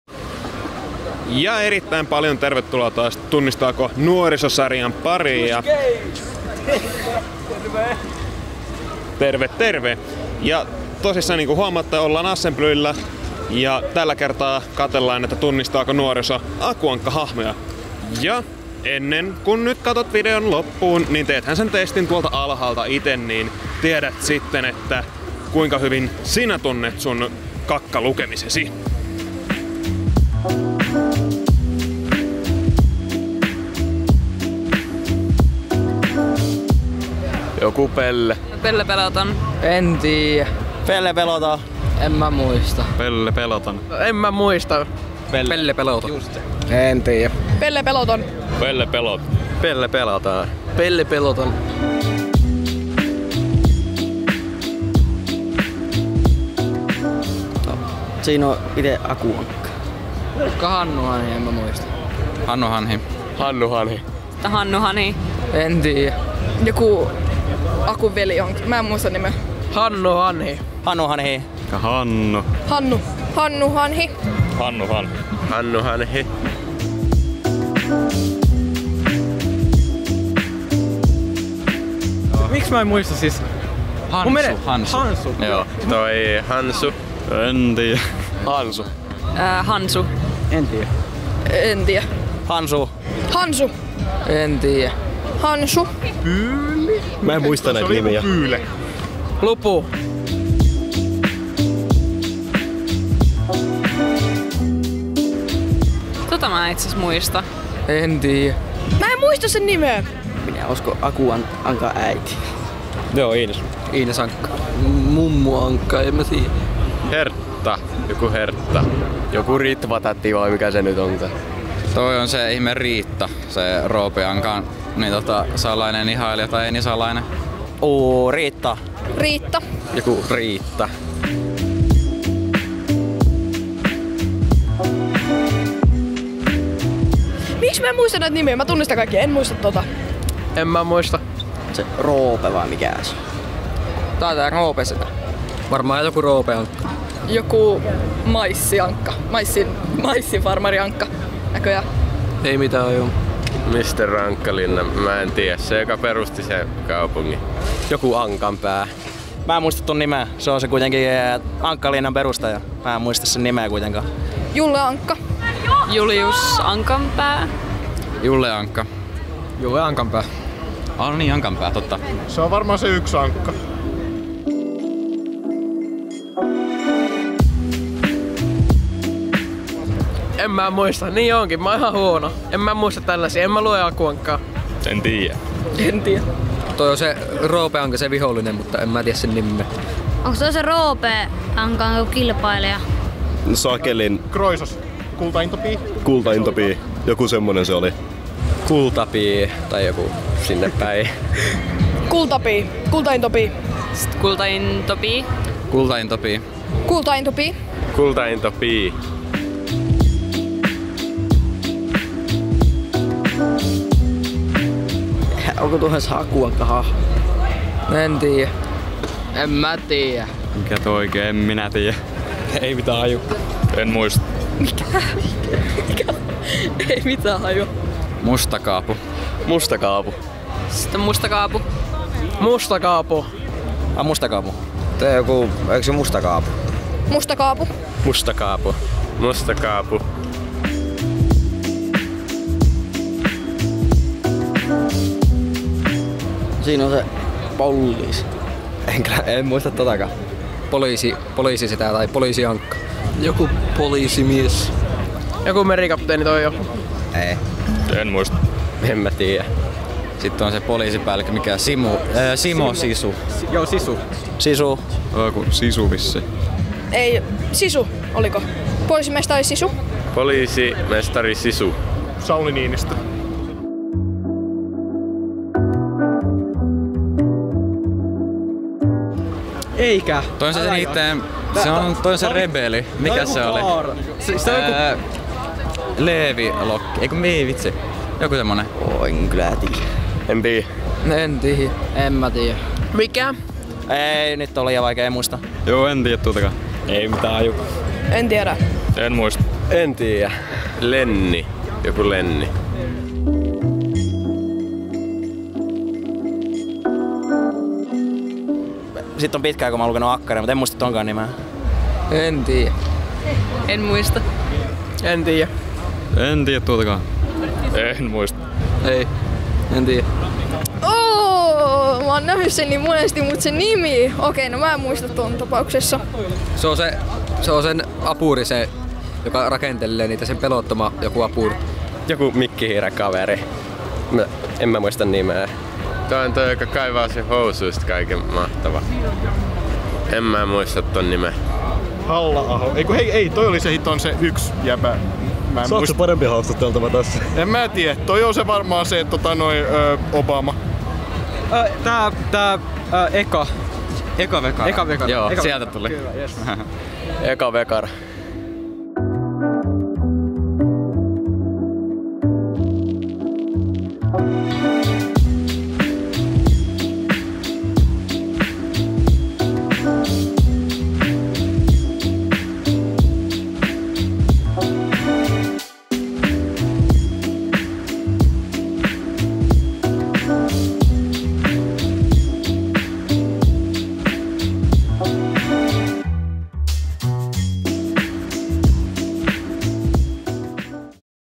Tunnistaako Nuoriso Aku Ankan -hahmot?
Hoxhox! Jos haluat tietää miten itse pärjäisit, tee testi täällä